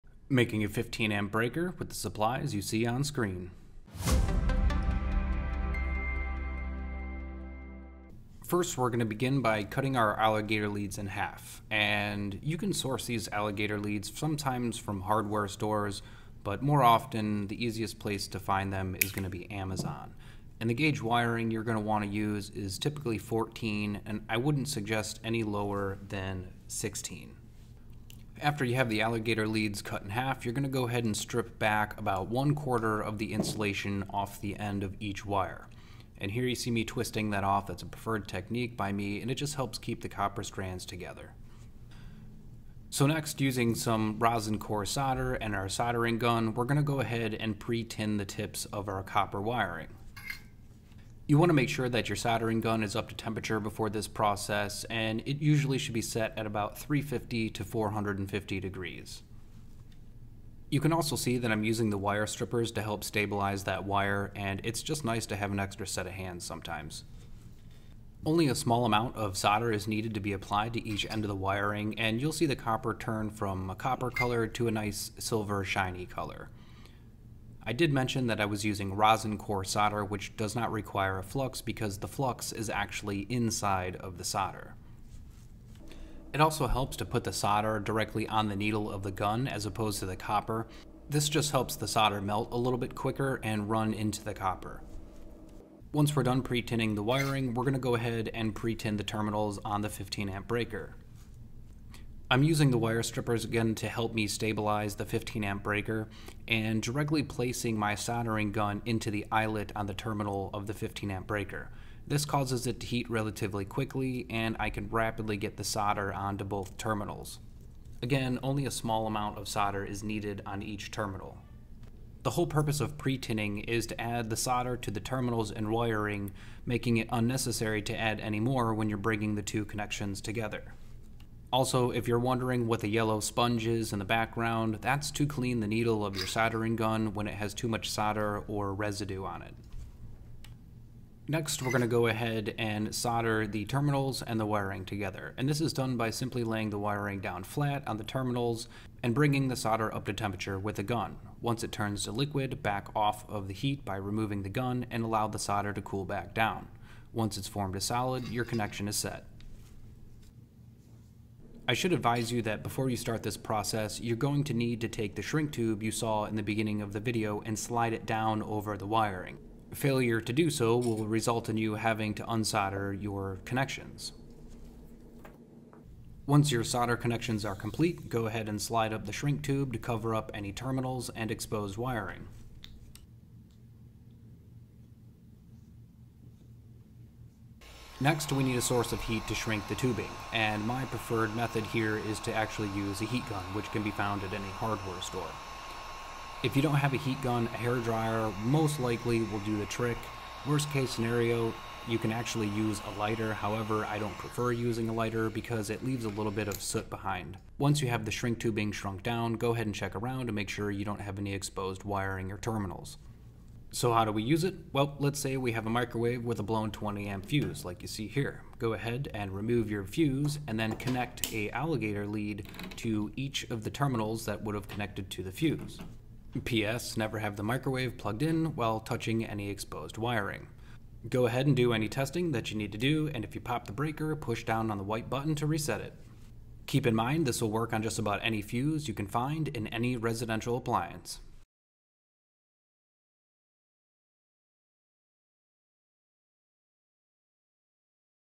How to make a 15-amp breaker
In this video, we will show you how to make your own 15-amp breaker.

‼️Make sure to take the necessary precautions before starting this project.